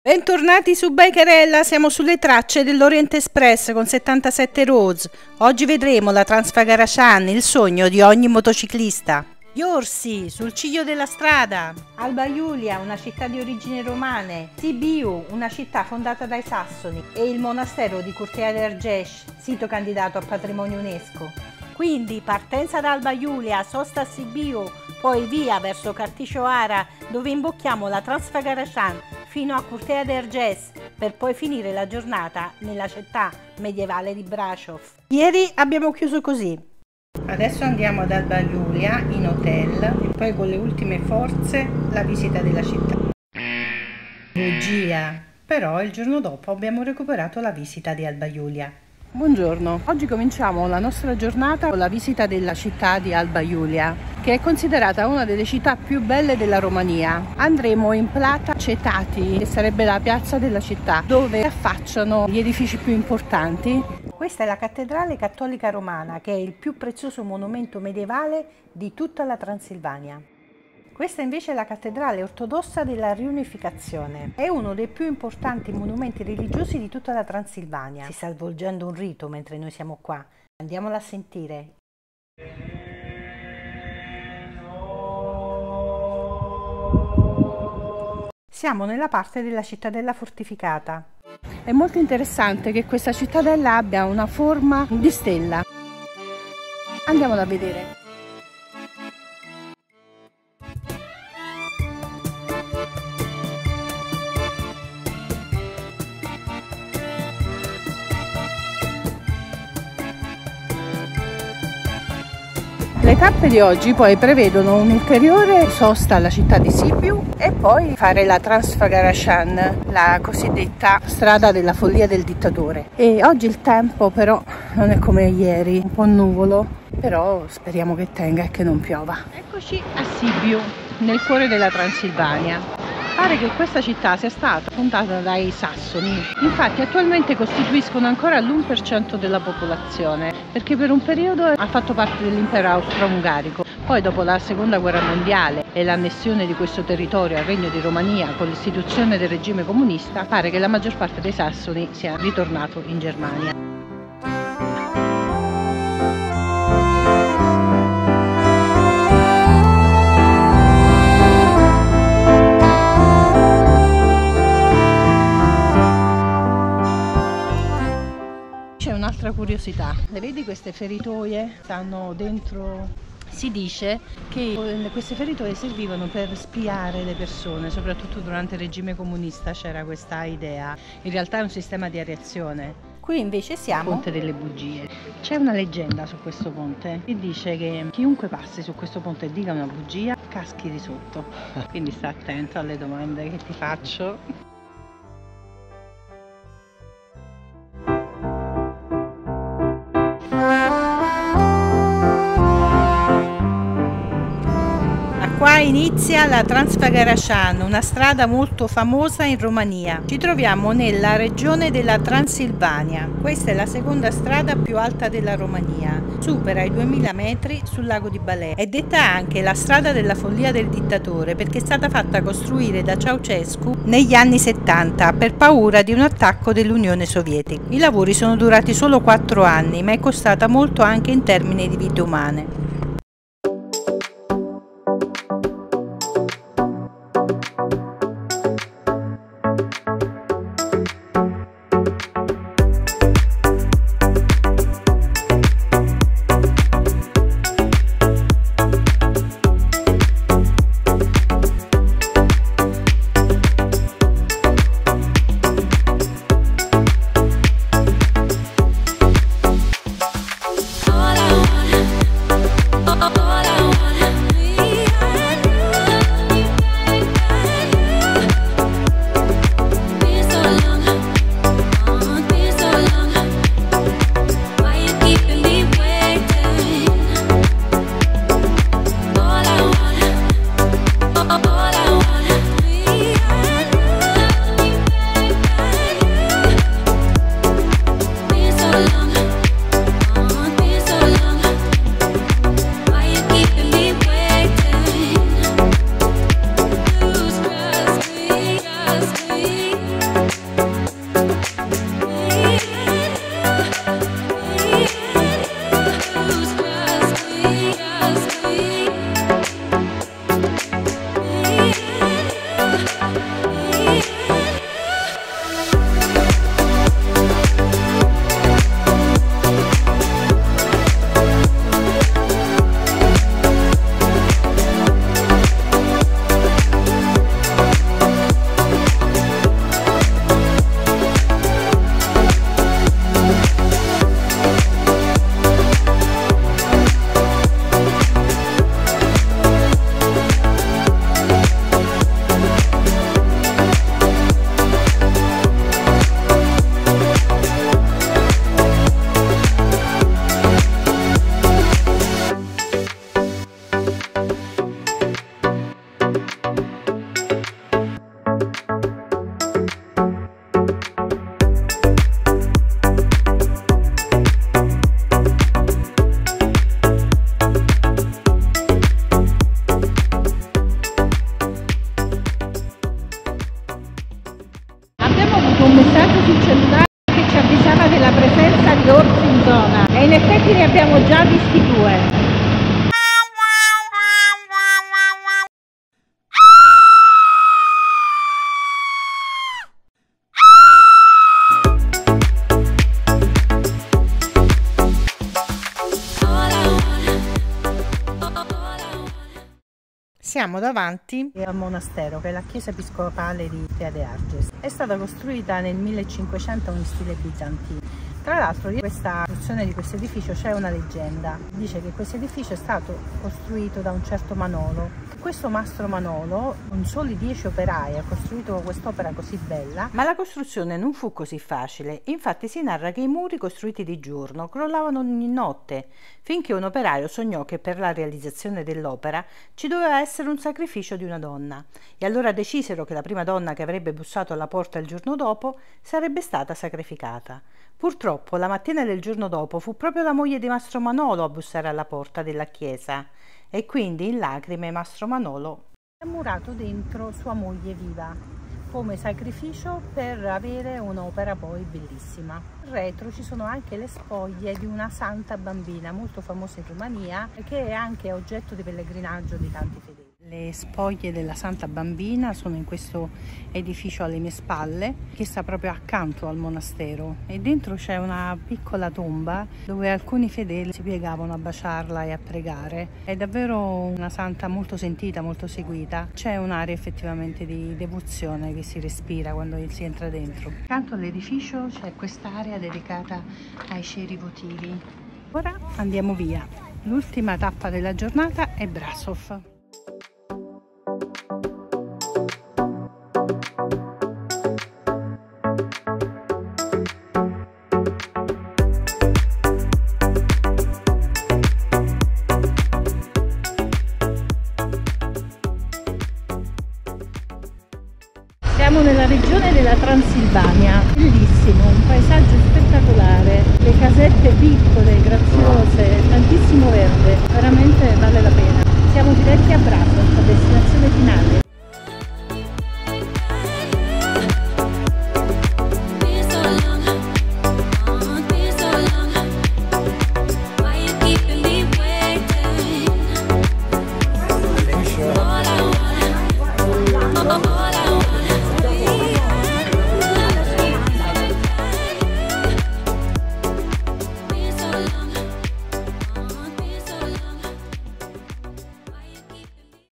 Transfagarasan Romania in Moto Orient Express con @77roads_viaggiinmoto ORSI! CHE PAURA!
Îl 3 episodio del nostro viaggio in moto con @77roads_viaggiinmoto  Transfagarasan Alba Iulia Sibiu. Bellissima scoperta: La Romania